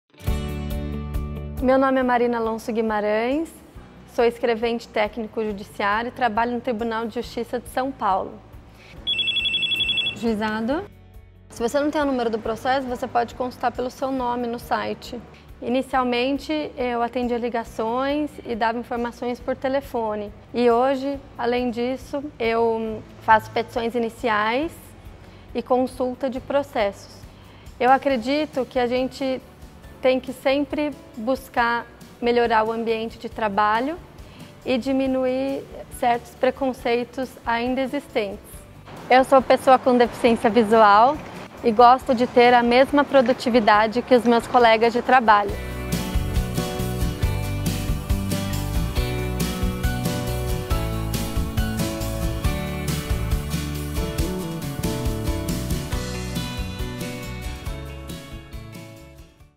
1ª Semana de Acessibilidade / Campanha Inclusão é Valorizar
Vídeo com a servidora Marina Alonso Guimarães, escrevente do Juizado Especial Cível Central